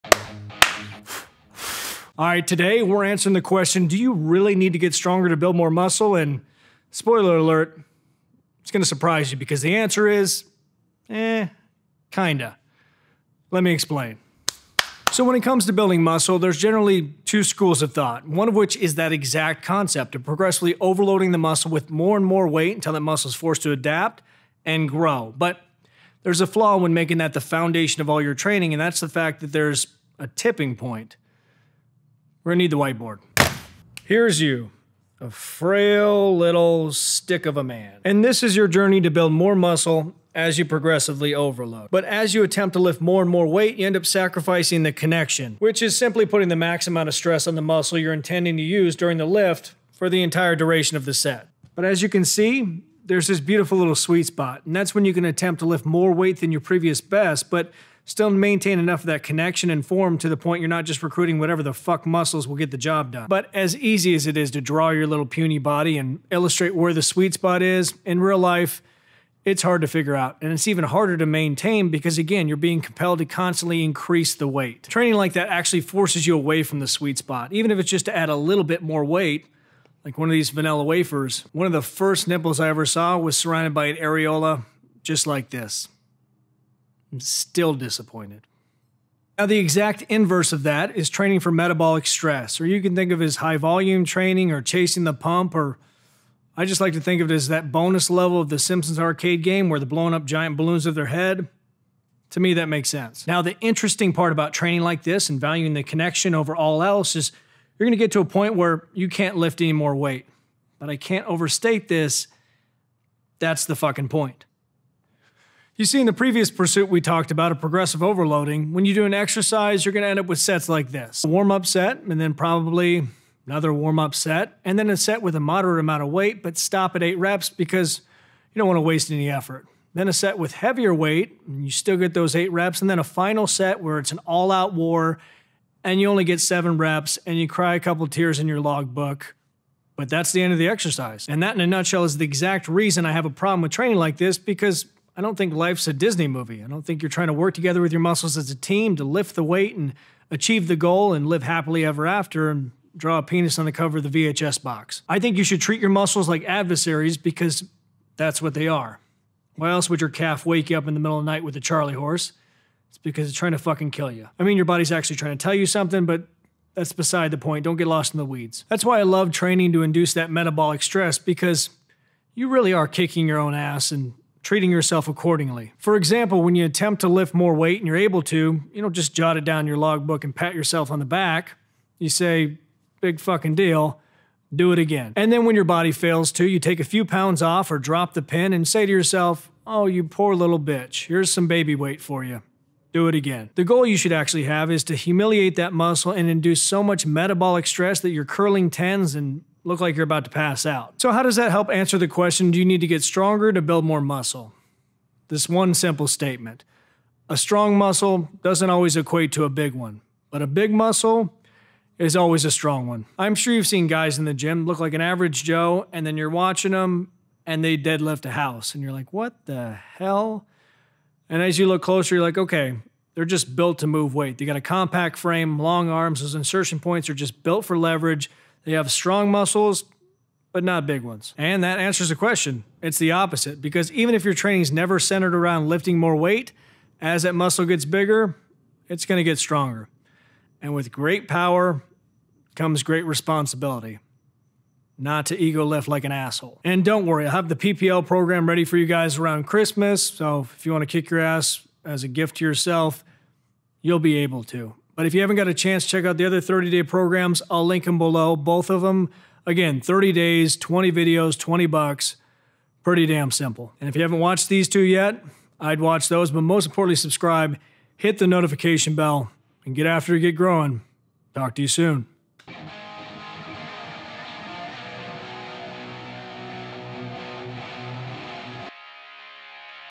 Do You Need To Get STRONGER To BUILD MORE MUSCLE? (MAYBE!)
Alright, today we’re answering the question do you need to get stronger in order to build more muscle?!

Well if you’re someone who progressively overloads then absolutely but if you train for the pain of metabolic stress then the answer is actually NO. But the funny thing is you will still end up stronger because a bigger muscle is ALWAYS a stronger muscle.